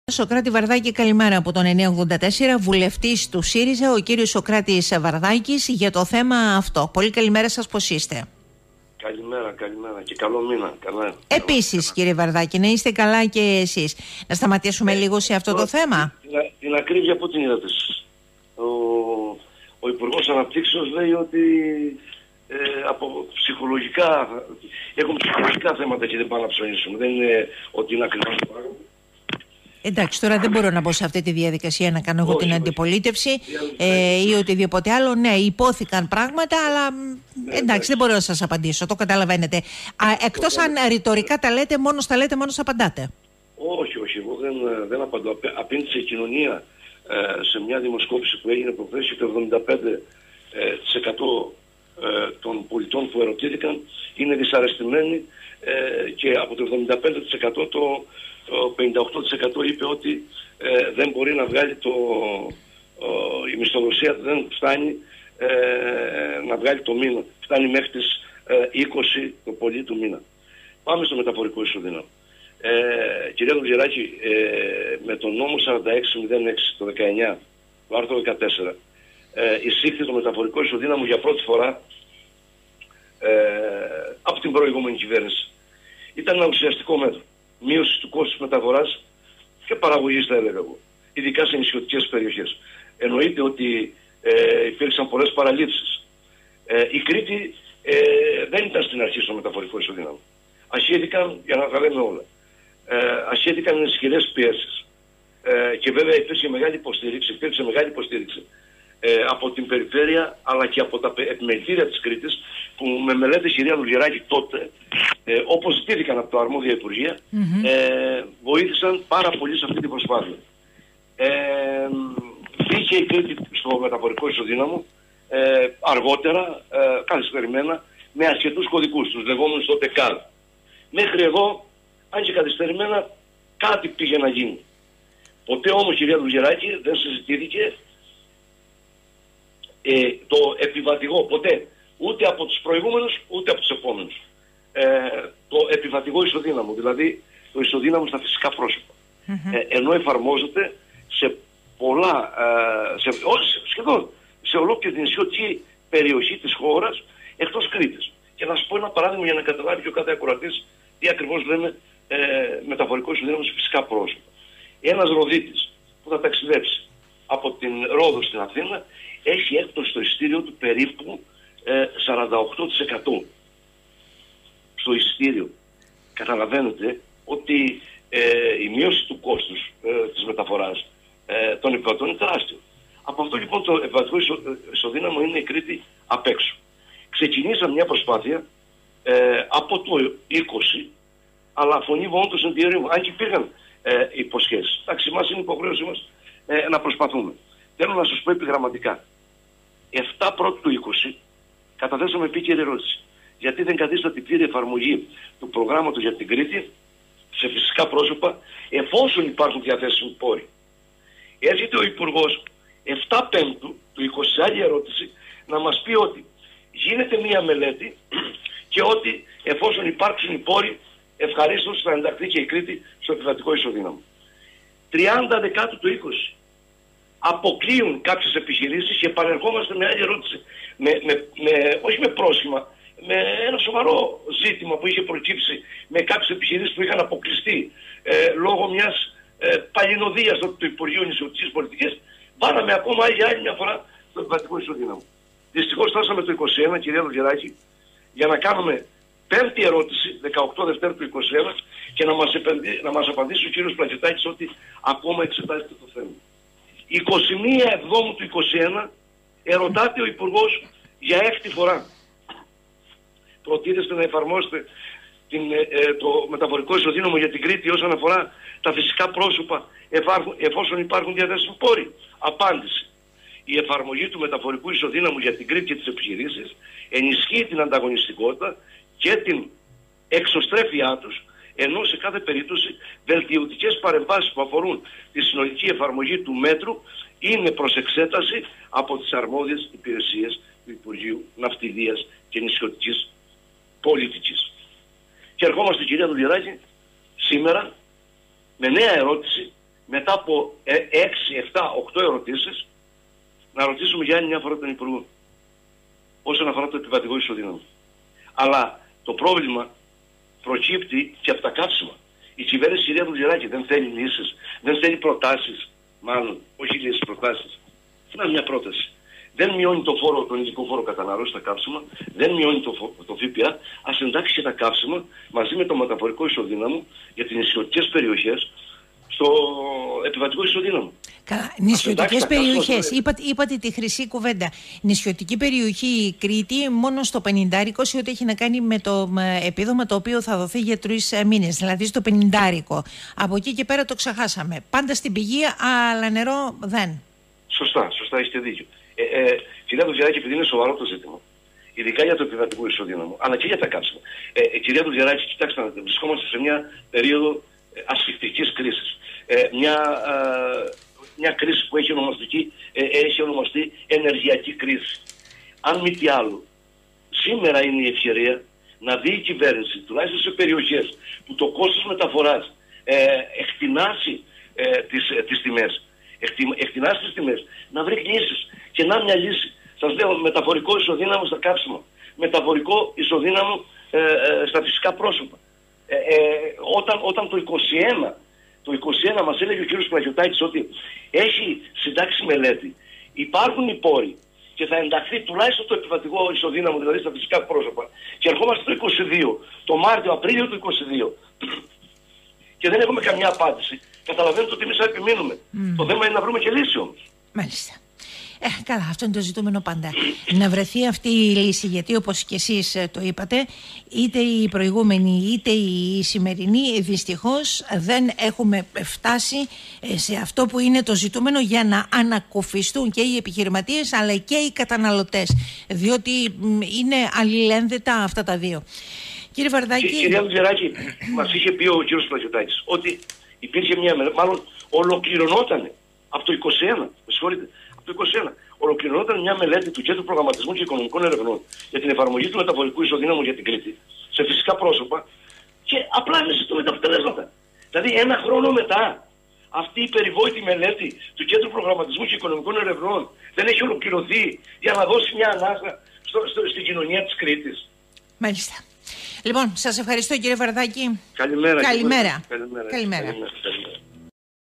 Σ. Βαρδάκης: Τώρα, η πλήρης εφαρμογή του Μεταφορικού Ισοδύναμου
Ανάγκη να στηριχθεί η νησιωτική Ελλάδα

Την πλήρη σε καύσιμα και φυσικά πρόσωπα, ζητούν από την Κυβέρνηση 46 Βουλευτές του ΣΥΡΙΖΑ με πρωτοβουλία των Σωκράτη Βαρδάκη και Νεκτάριου Σαντορινιού, καταθέτοντας σχετική ερώτηση προς τον Υπουργό Ναυτιλίας και Νησιωτικής Πολιτικής.
«Η ένταξη της Κρήτης στο πρόγραμμα του Μεταφορικού Ισοδύναμου από την προηγούμενη Κυβέρνηση ήταν η απτή απόδειξη ότι ο ΣΥΡΙΖΑ είχε ένα ουσιαστικό σχέδιο ανάπτυξης για τα νησιά, με ουσιαστική ενίσχυση στην κατεύθυνση της τόνωσης της ανταγωνιστικότητας των επιχειρήσεων, με πολλαπλά έμμεσα και άμεσα οφέλη για την εθνική και περιφερειακή οικονομία» σημειώνει ο Βουλευτής, αναφέροντας με ημερομηνίες τις κινήσεις που είχαν γίνει στο πλαίσιο αυτό.
Στην ερώτηση τους οι Βουλευτές αναφέρουν πως οι συνθήκες επιβάλλουν την επαναφορά του μέτρου και την επέκτασή του σε όλα τα νησιά, μεταξύ των οποίων και η Κρήτη.